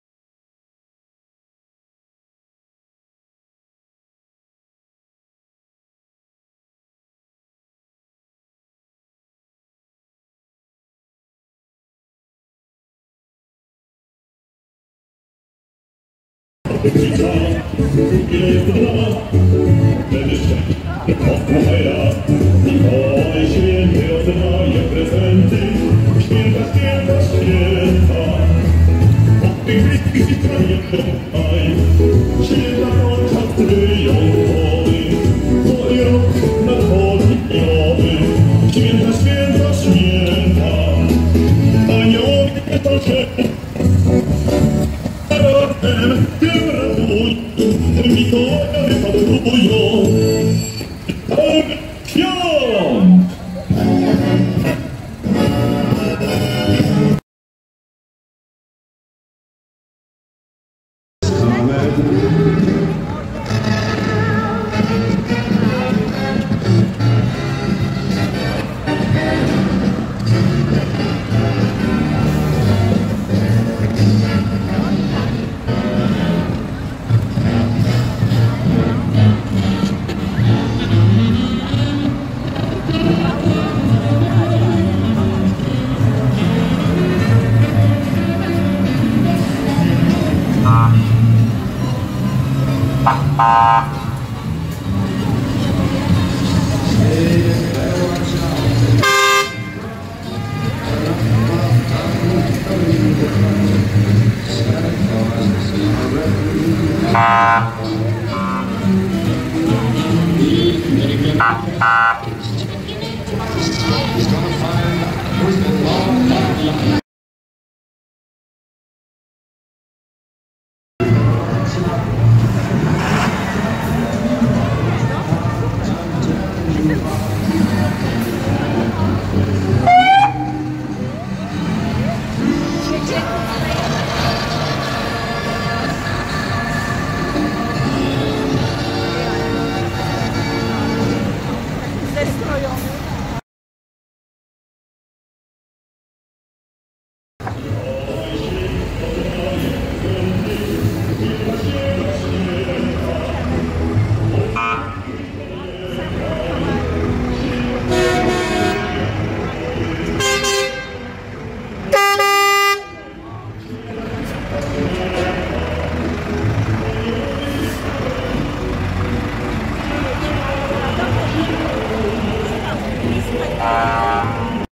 IV Pabianicka Parada Świąteczna
Nagranie z IV Pabianickiej parady świątecznej w której brały udział :
Ochotniczy Sztab Ratownictwa Pabianice
489-65, 68 - OSP w Chechle
489- 70 , 72 - OSP Pabianice